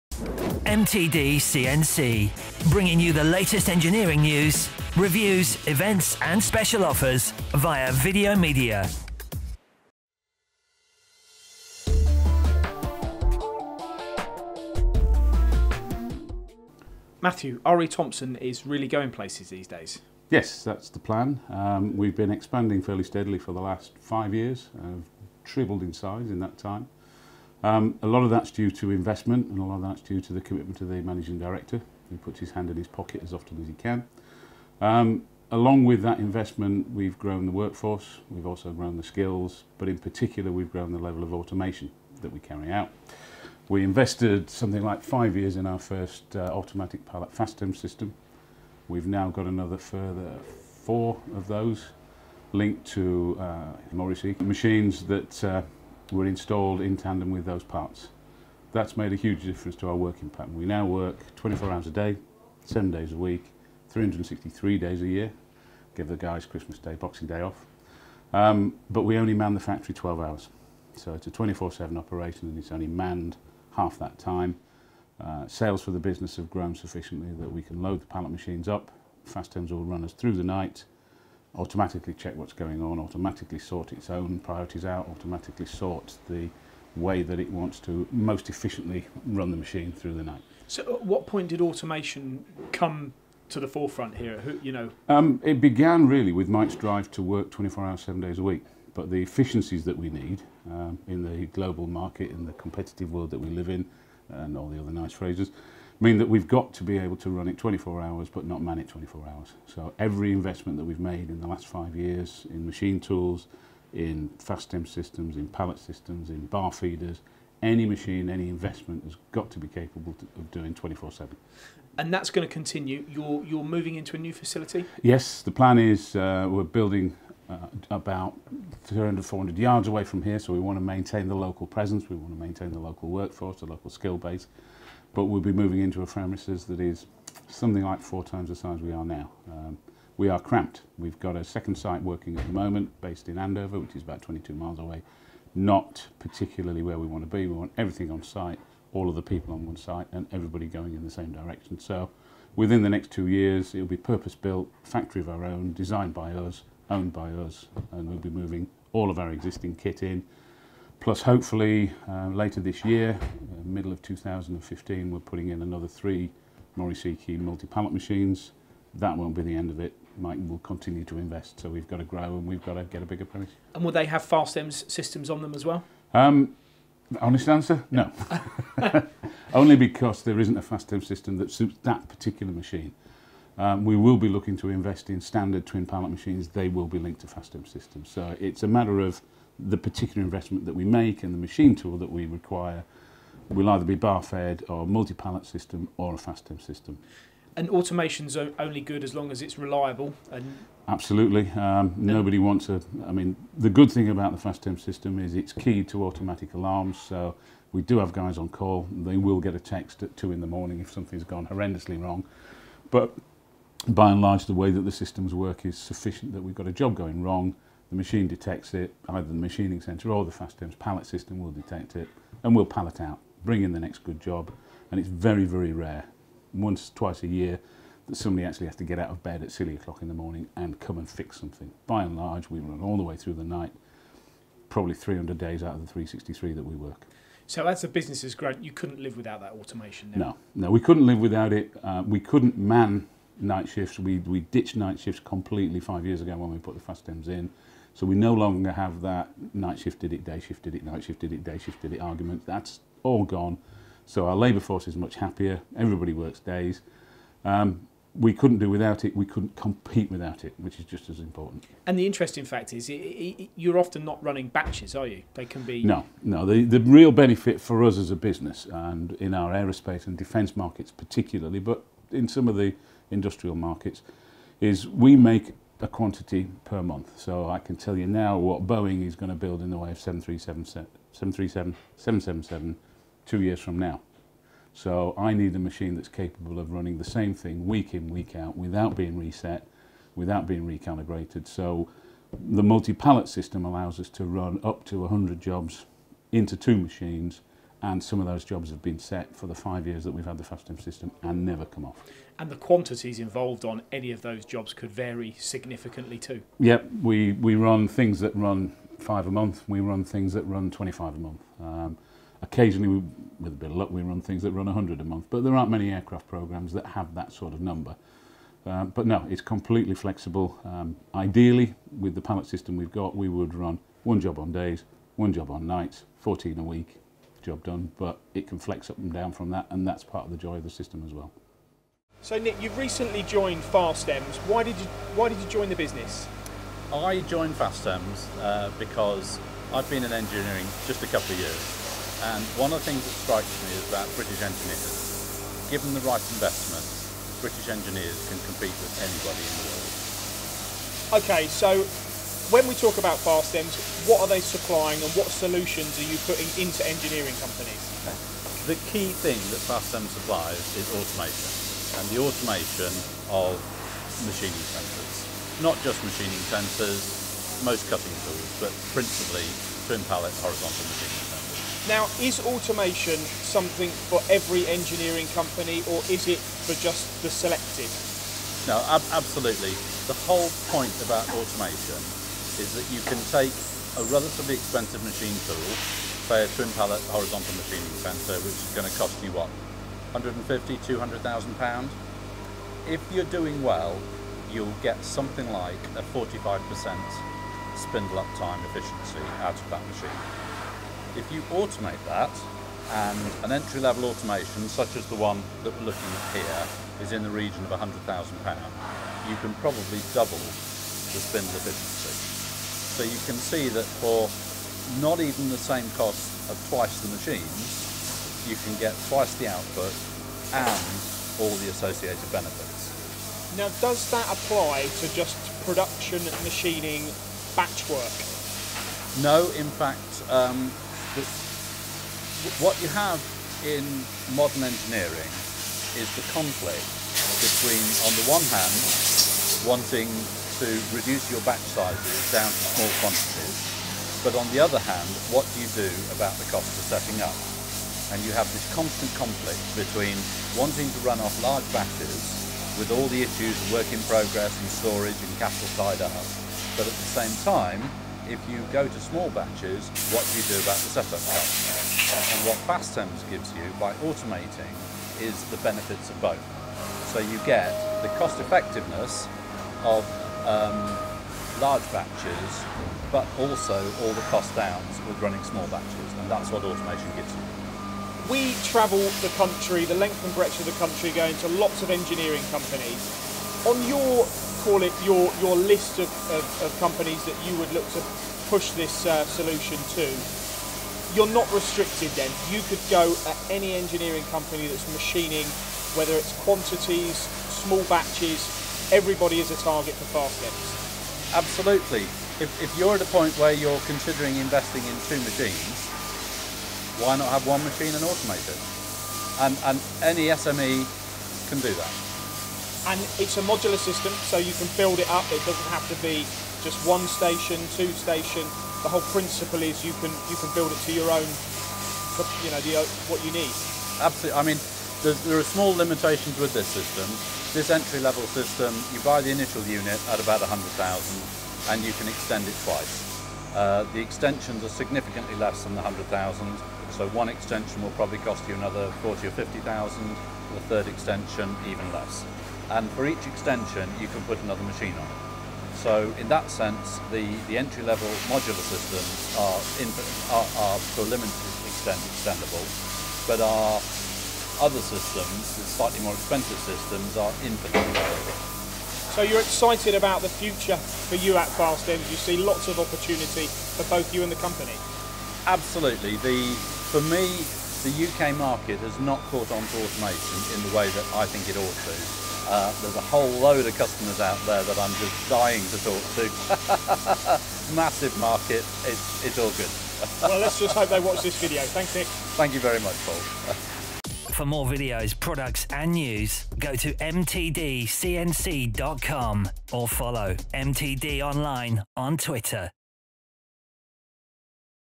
R E Thompson endorse FASTEMS automation
R E Thompson are a well respected and established subcontractor in the south of England who put some of their success down to the automation systems from FASTEMS.